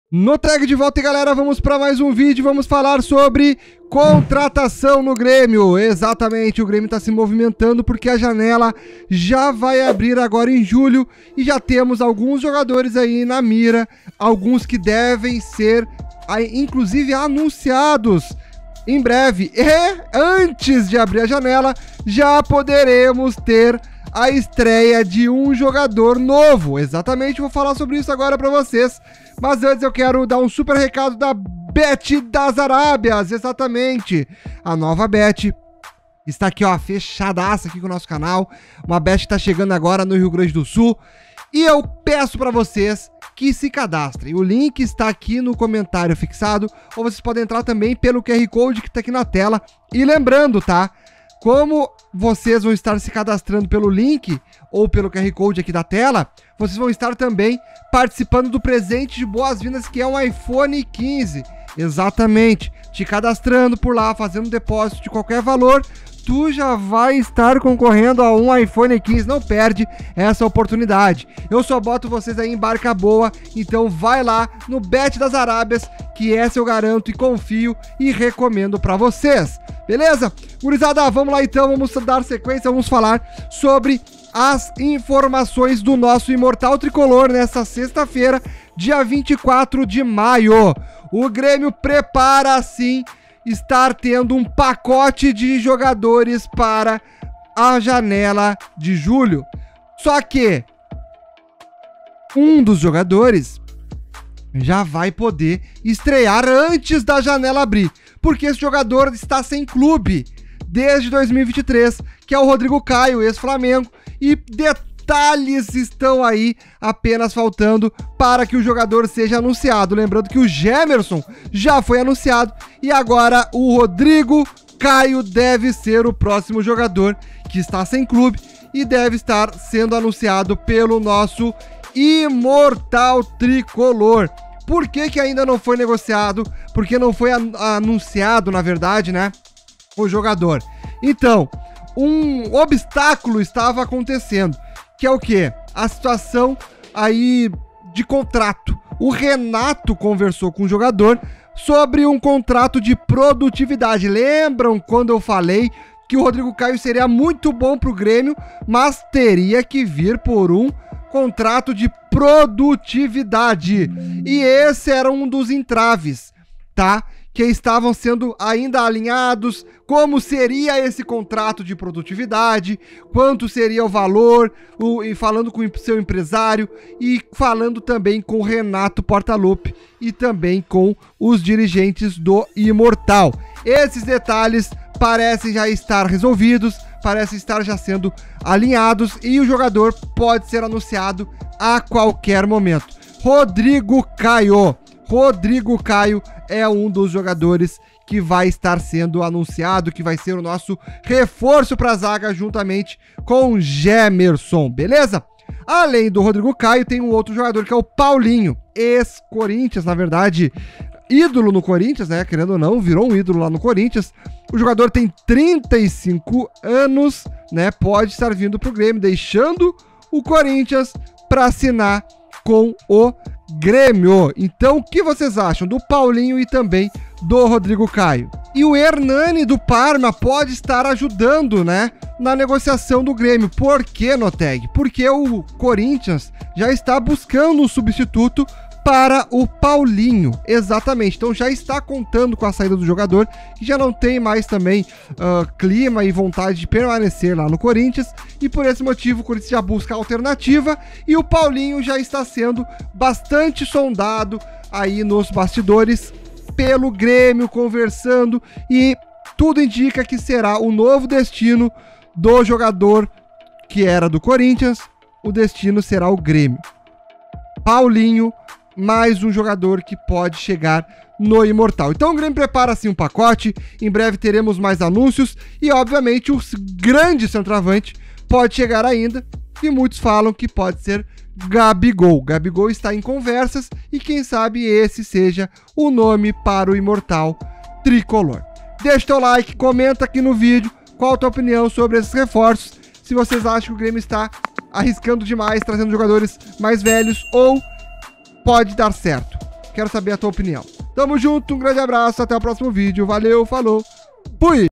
🚨 NO DETALHE! PRÓXIMO DE ANÚNCIO, NOVOS REFORÇOS NO GRÊMIO! RODRIGO CAIO, PAULINHO E GABIGOL!
Grêmio está negociando com jogadores e próximo de anúncios!